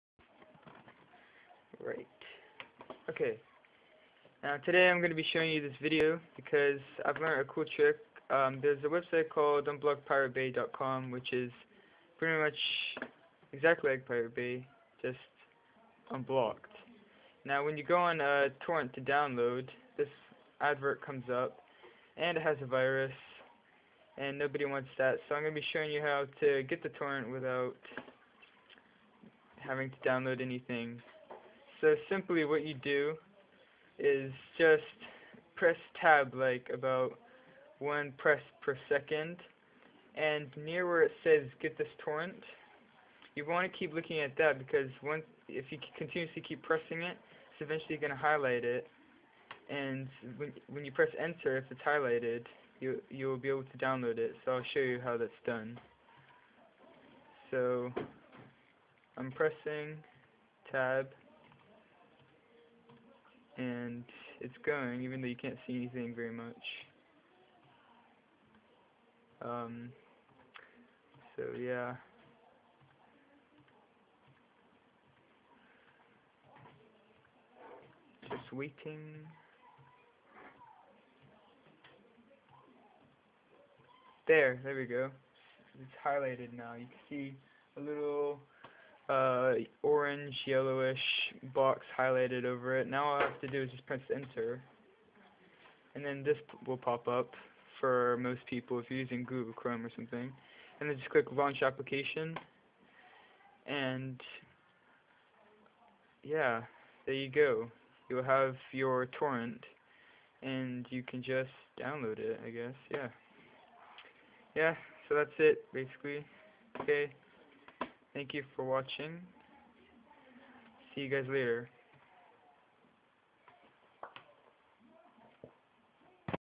How to get past ads and download files
Alright what you do is.

1. Go to the page with the advert blocking the download.

2. Make sure you can see the download button that you need to press.

3. Continually press tab until the download button is highlighted(don't press to fast!).

4. When it's highlighted press enter and there you go.

note: works for for almost every site with ads